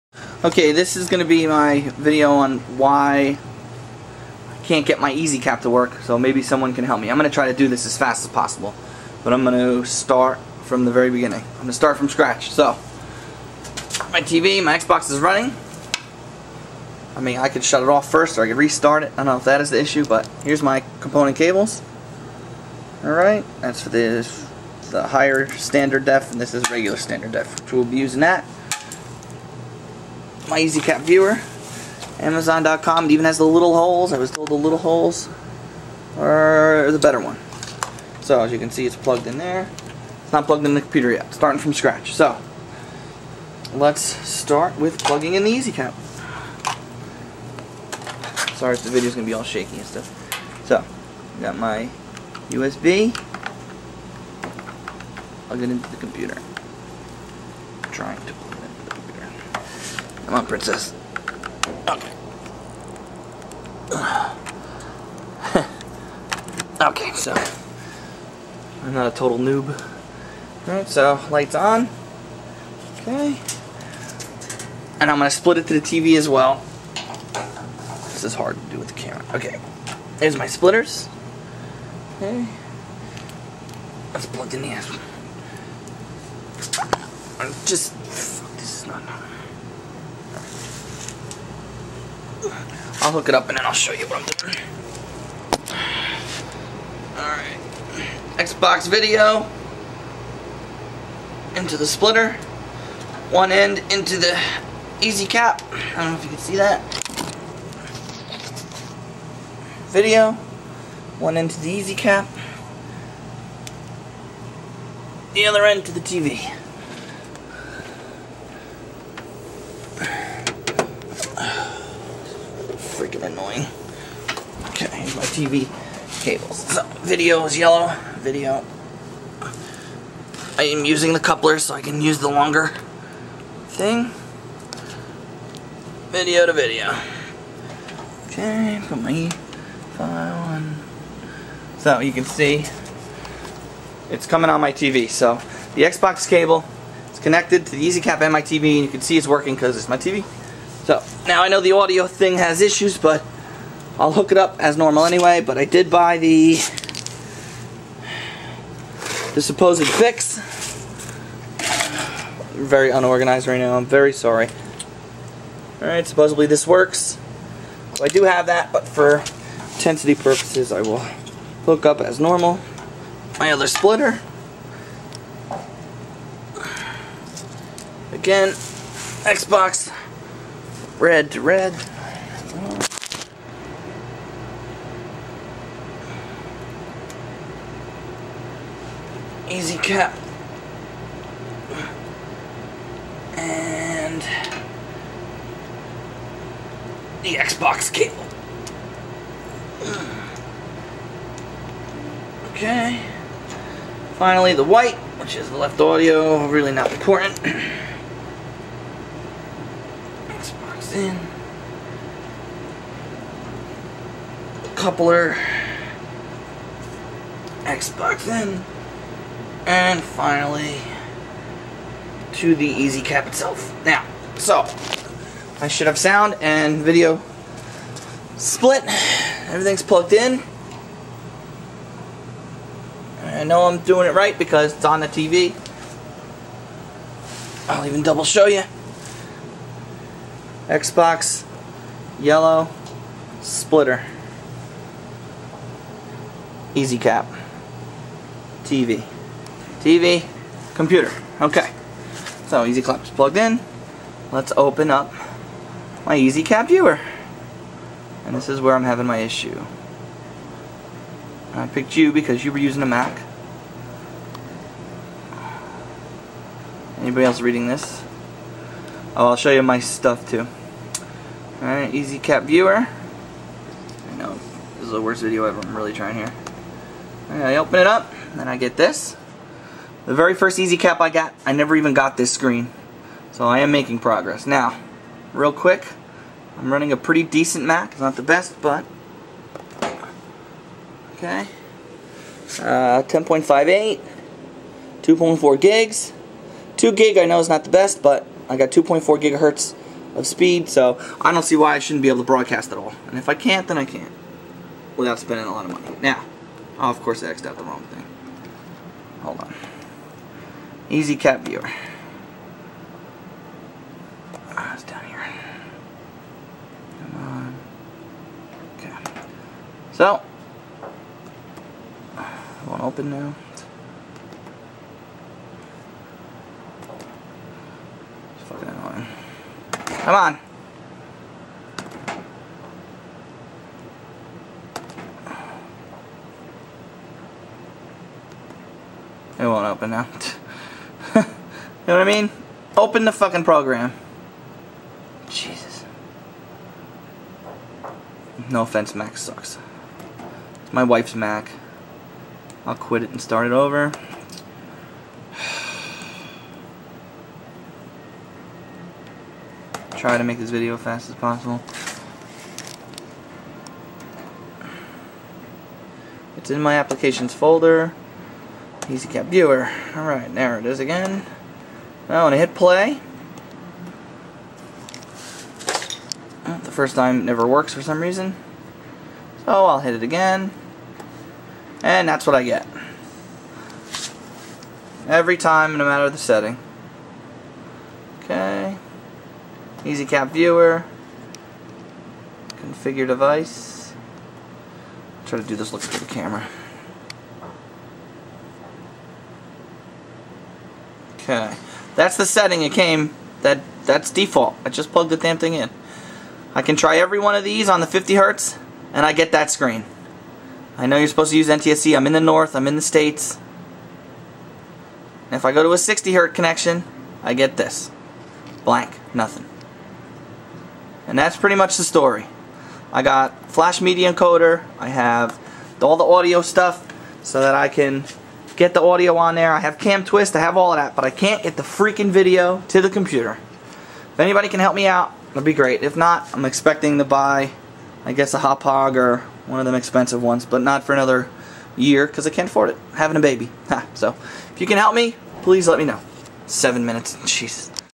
easycap problems
Why doesnt my easycap view work??? help me please